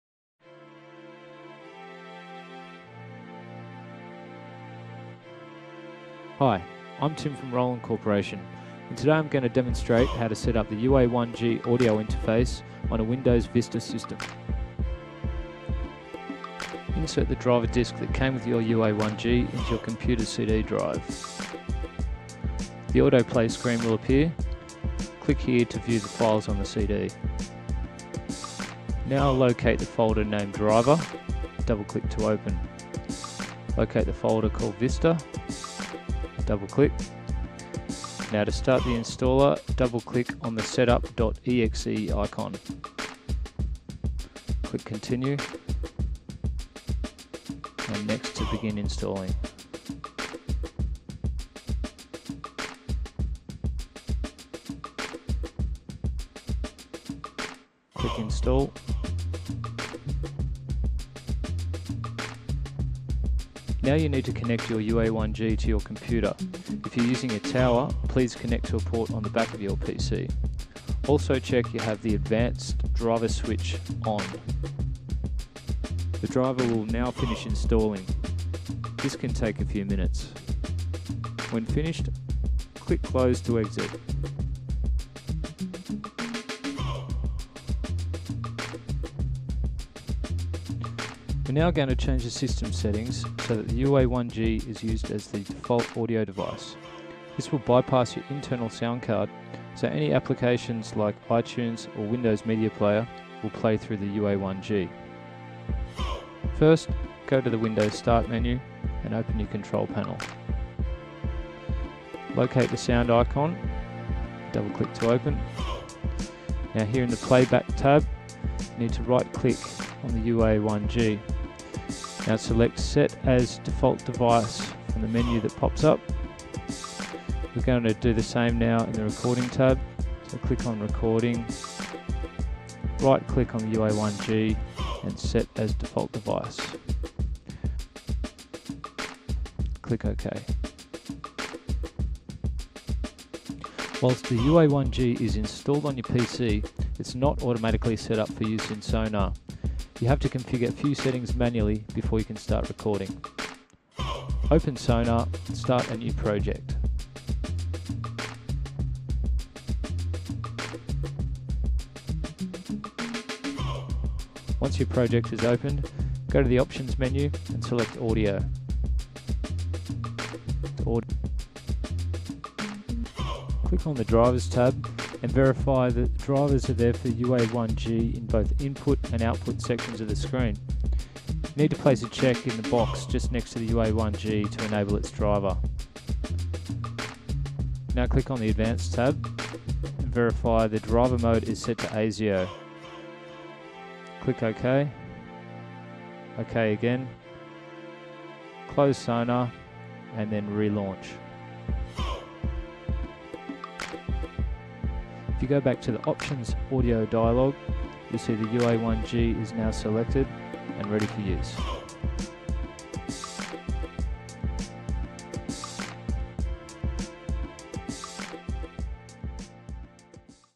Cakewalk by Roland Audio Interface Installation Tutorial
Step by step tutorial to install a Cakewalk by Roland USB Audio Interface on a Windows Vista computer.